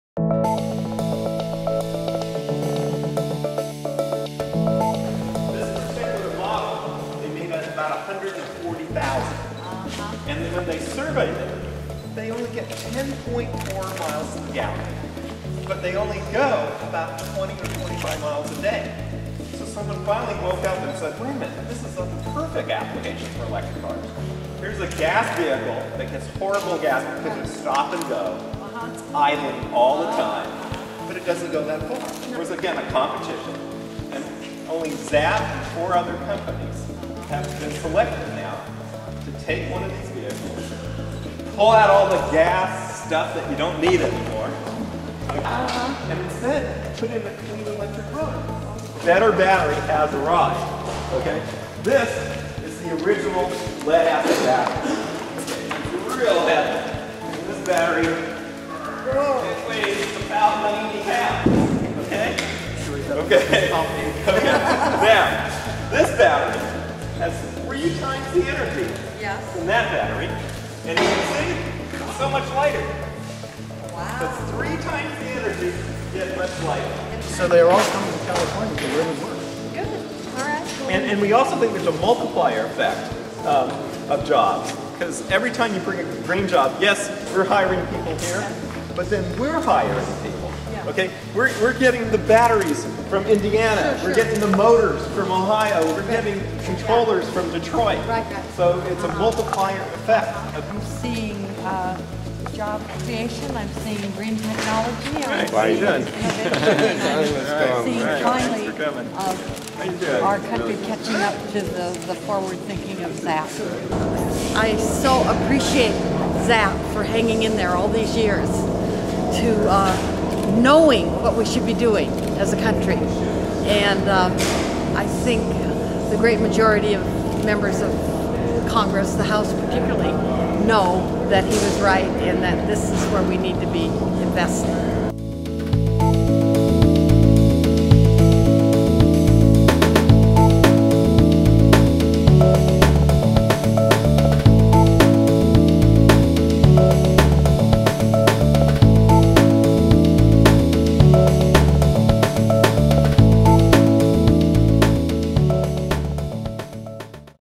US Congresswoman Lynn Woolsey Tours ZAP Electric Postal Vehicle Project in Santa Rosa, California
US Postal Service Awards ZAP Engineering Contract for Electric Mail Truck

SANTA ROSA, Calif., Feb. 17 /PRNewswire-FirstCall/ — The United States Postal Service (USPS) has awarded electric car pioneer ZAP (OTC Bulletin Board: ZAAP) an engineering contract for the design and development of an electric version of its Long Life Vehicle (LLV).
Congresswoman Lynn Woolsey visits ZAP

ZAP will convert the gasoline mail truck to run on electricity as specified by the USPS at facilities in Santa Rosa, California. Following completion the converted vehicle will undergo field trials in the Washington, DC metropolitan area later this year. ZAP was selected along with four other firms in a competitive bid process held nationwide by the USPS.

Yesterday, US Congresswoman Lynn Woolsey, D-California, toured ZAPs facility, commenting on job creation by ZAP and the Postal Service: "I so appreciate ZAP for hanging in there all these years, knowing what we should be doing as a country. I think a majority of members of Congress, the House in particular, know that they were right and that this is where we need to be investing."